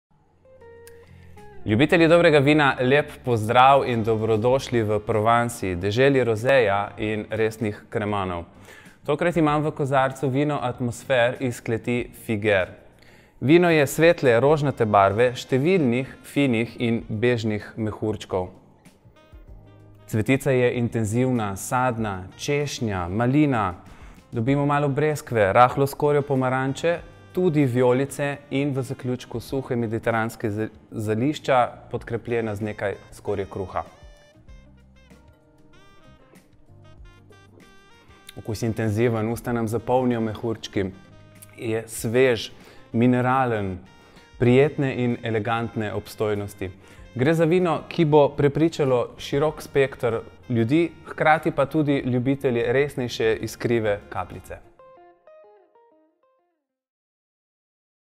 Opis vina s sommelierjem: Atmosphere Figuiere
Sommelier 3. stopnje Valentin Bufolin nam predstavi provansalski peneči rose Atmosphere iz kleti Figuiere.